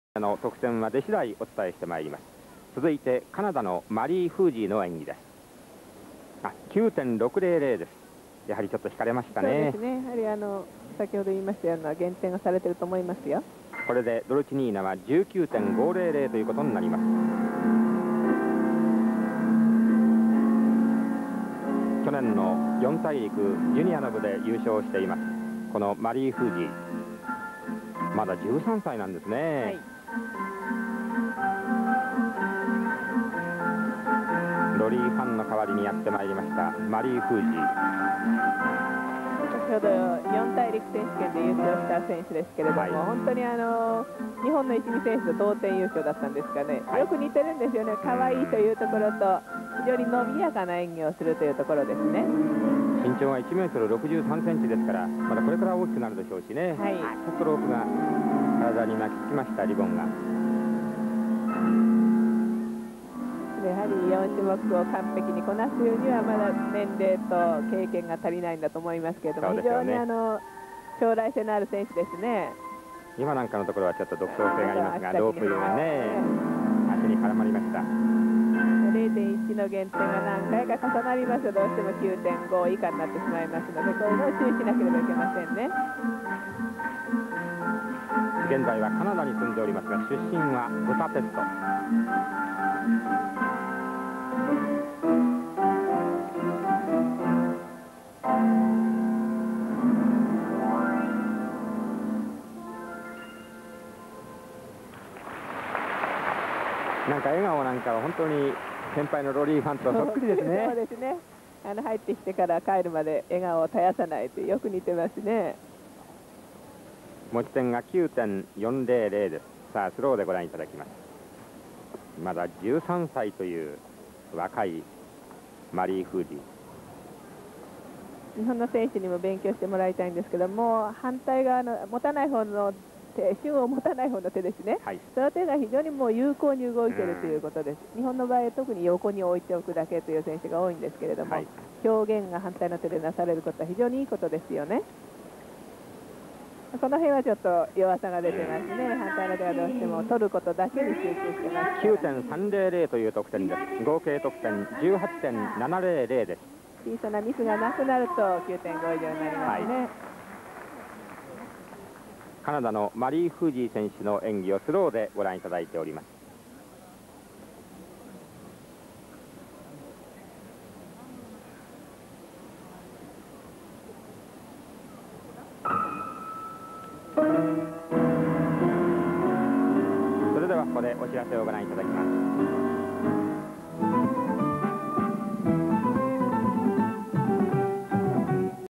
Mary FUZESI (CAN) ribbon - 1987 Brother Cup EF
Mary FUZESI (CAN) - Ribbon final

'87 Brother Cup in Tokyo